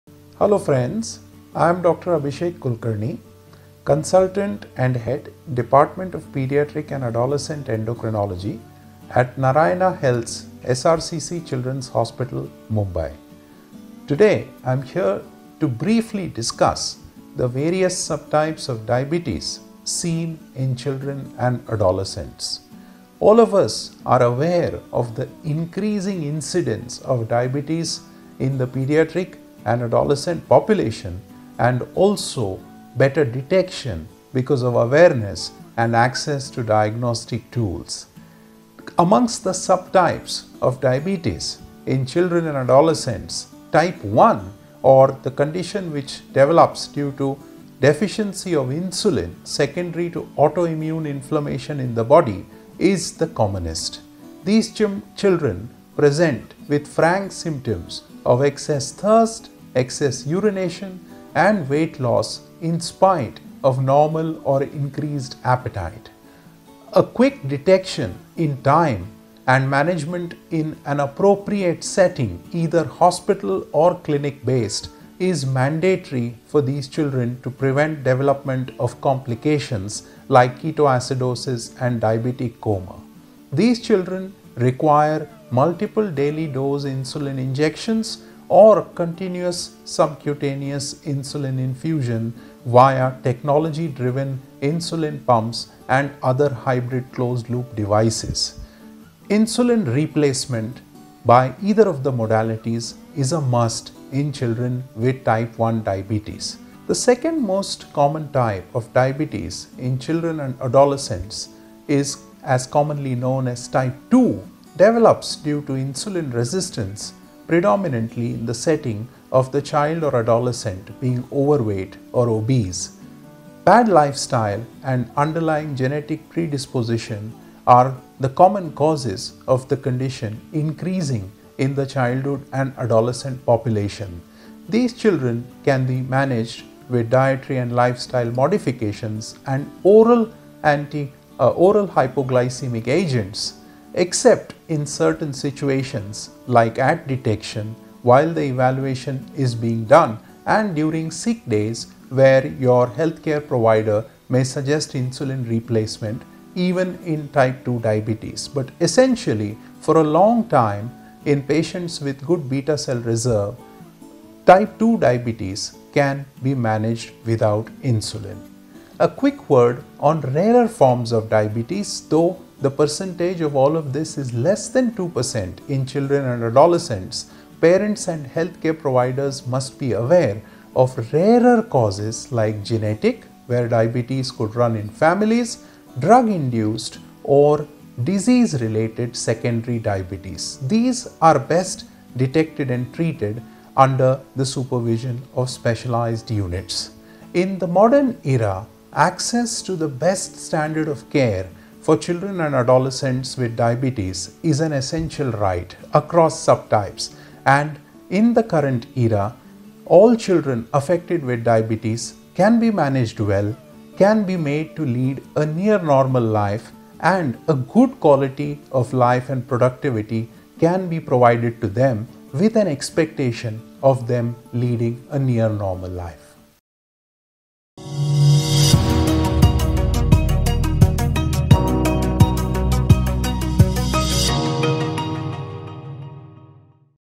World Diabetes Day: Types/Subtypes Of Diabetes & How To Control Diabetes Problem | TimesXP
Watch Dr. Abhishek Kulkarni, Consultant – Paediatric Endocrinology, SRCC Children’s Hospital, managed by Narayana Health, explain the different types and sub types of diabetes and various methods that help control it.